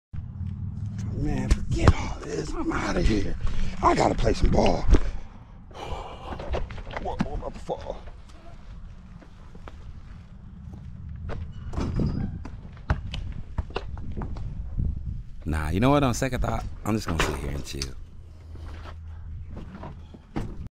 SiT here & CHiLL | Bike 🚲 Shot 🏀
When your on Quarantine but you want to ride 🚲 & play basketball 🏀

Gear:
GoPro HERO 8 Black
GoPro MAX
Extra Battery
SanDisk Extreme 64GB microSDXC

128 GB SanDisk Extreme 64GB microSDXC

Jesus is WORTHY to be PRAiSED!!!